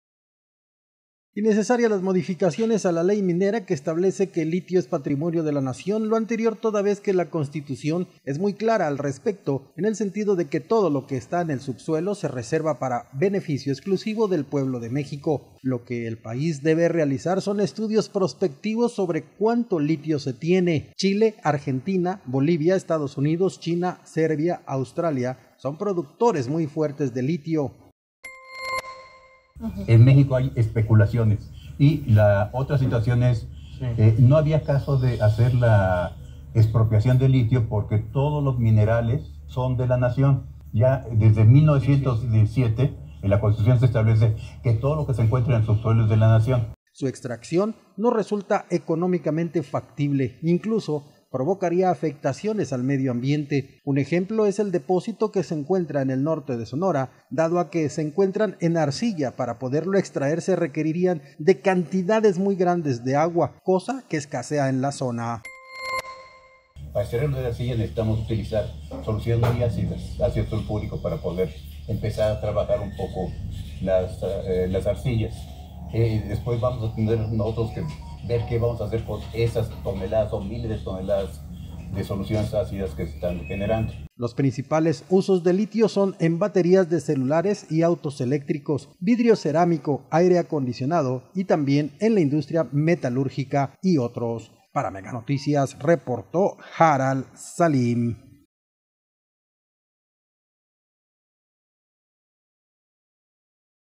El litio ya estaba establecido en la constitución dicen especialistas, los diputados puro circo
Extracción de litio provocaría afectaciones al medio ambiente